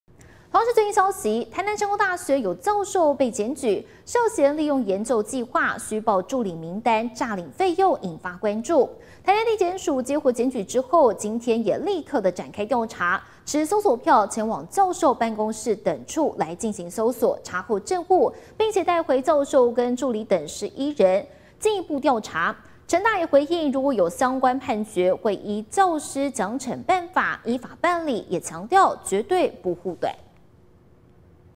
成大爆詐領助理費 檢進校搜索帶回教授等11人
台南成功大學，有教授被檢舉，涉嫌利用研究計畫，虛報助理名單，詐領費用，引發關注。台南地檢署今天(14日)上午，接獲檢舉後，立即展開調查，持搜索票，前往教授辦公室等處進行搜索，查扣證物，並帶回教授與助理等11人名，進一步調查。成大也回應，若有相關判決，會依教師獎懲辦法，依法辦理，強調絕不護短。